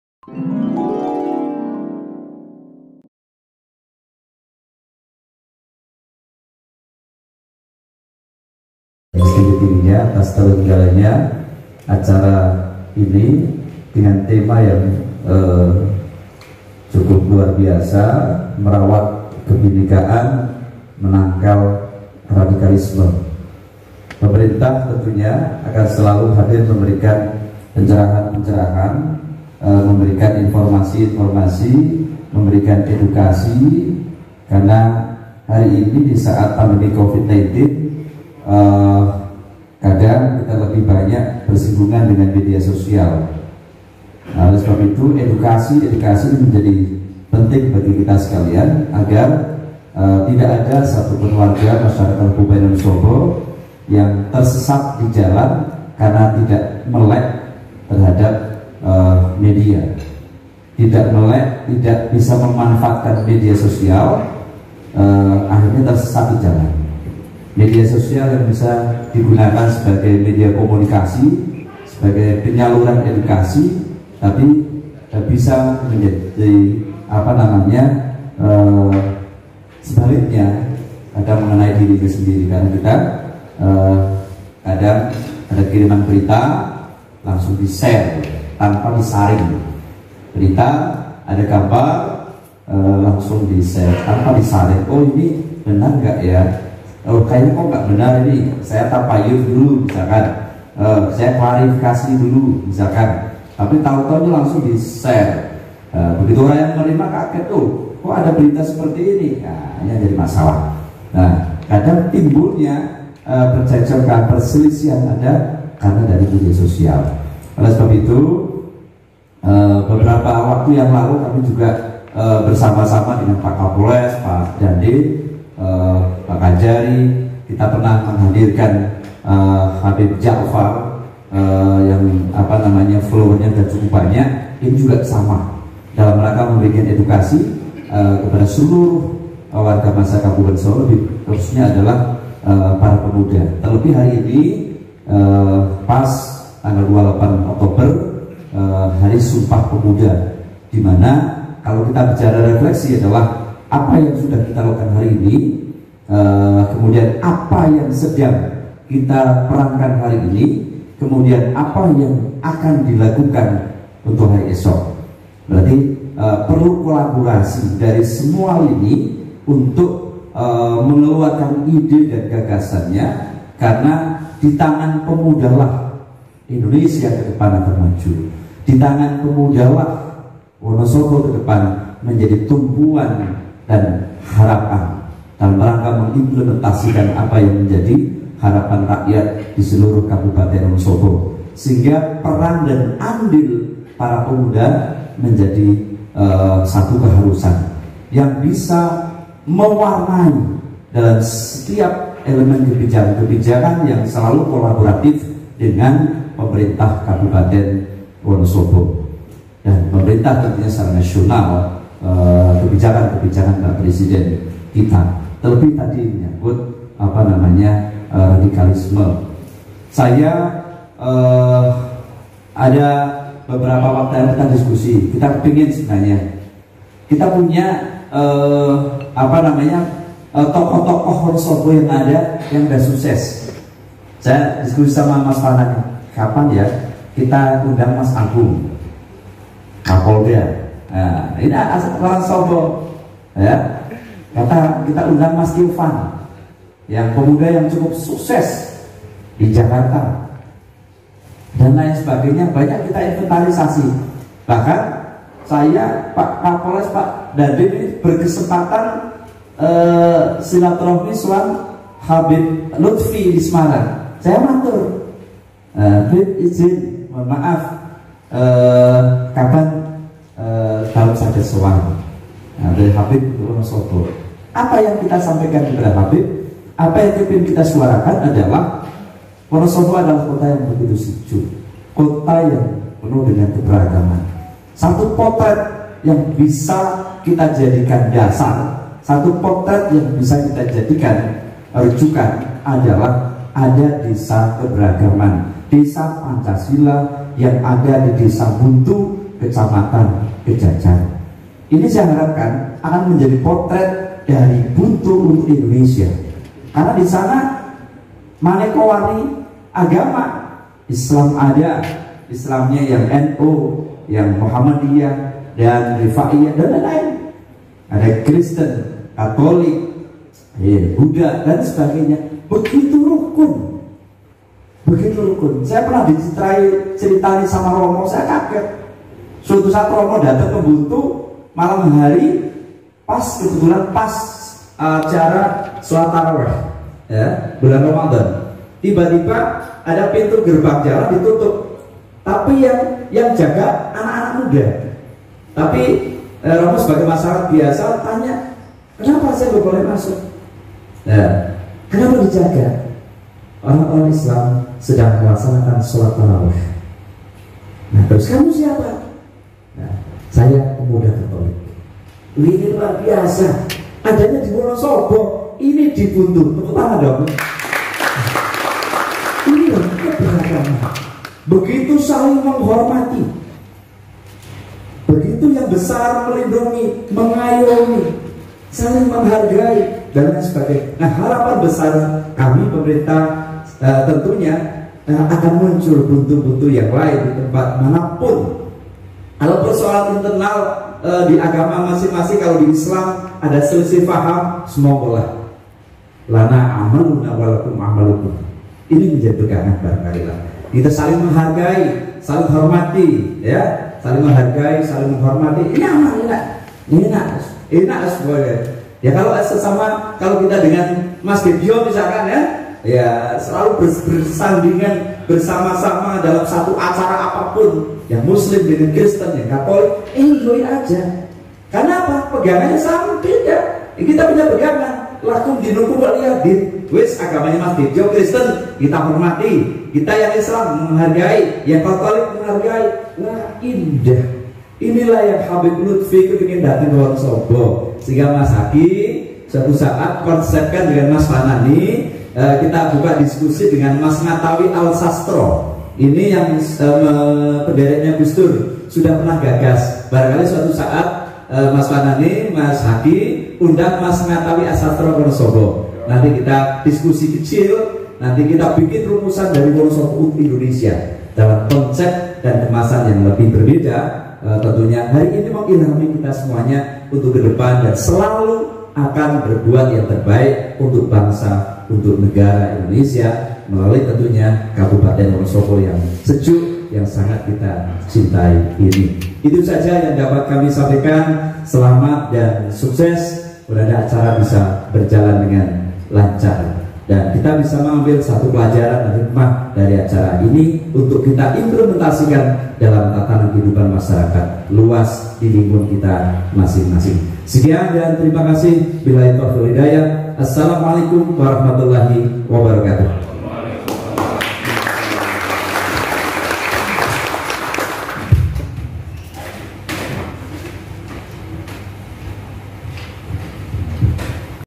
DESA BUNTU KEJAJAR PALING HUMANIS SEDUNIA ANDALAN WONOSOBO RAMAH HAM
Pidato Sambutan Bupati Wonosobo dalam Acara Sarasehan dan Refleksi Hasi Sumpah Pemuda yang diselenggarakan oleh KNPI Wonosobo.